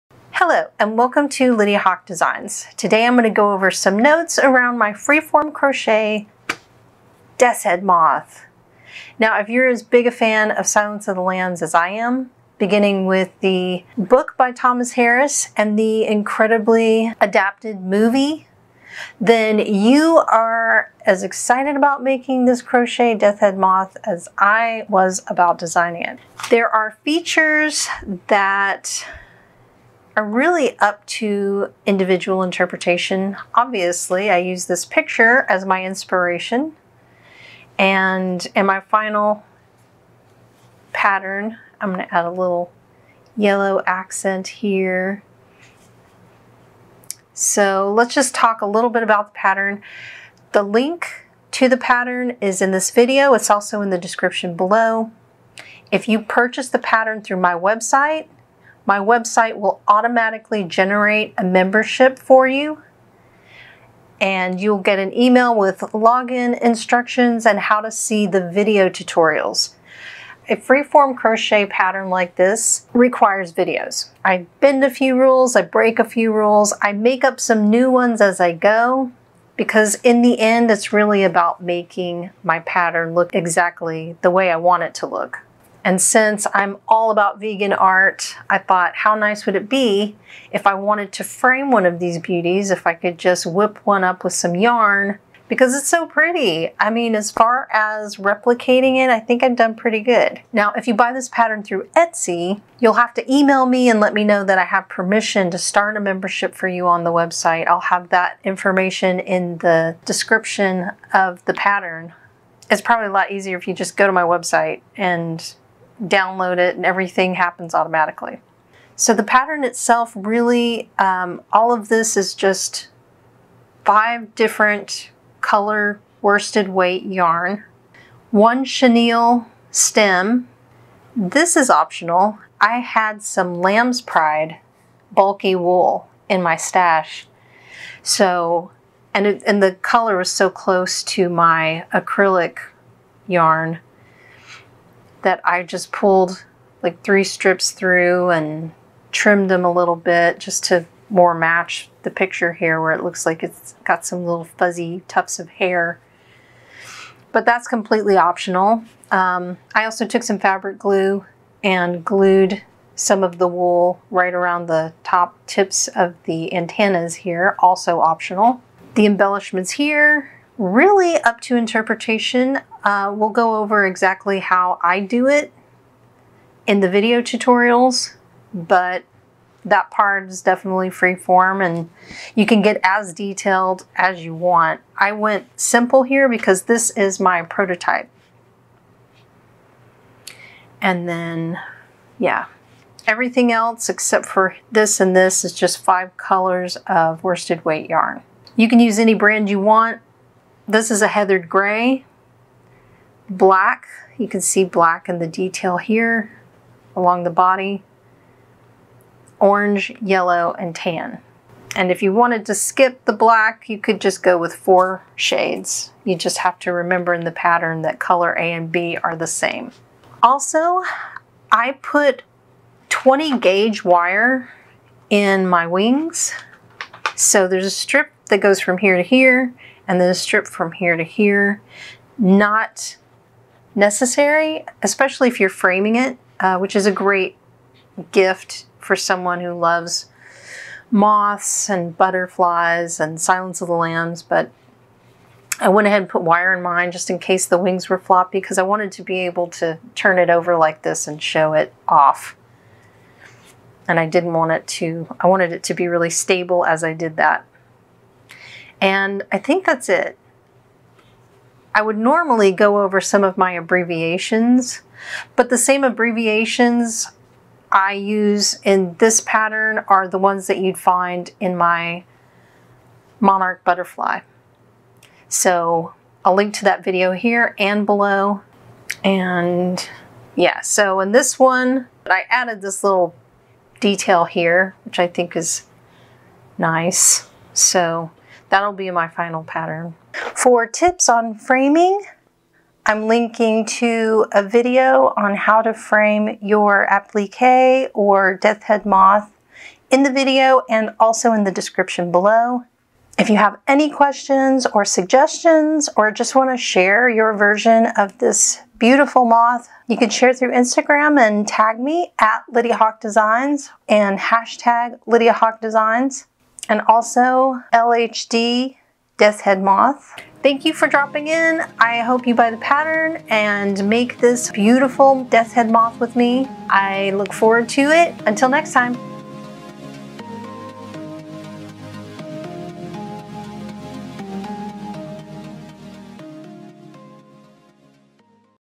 LHD Crochet Death's Head Moth: Introduction
Another labor of love, the LHD Freeform Crochet Death’s Head Moth is a gorgeous finished moth you can frame, gift and otherwise have a lot of fun making along with my step-by-step video tutorials!

Challenging and fun, we start with the Body, then move on to the Top Wings, then the Bottom Wings, then Assembly.

Your purchase will automatically generate a membership at no extra cost, with an email that explains how to download the pattern and access the videos.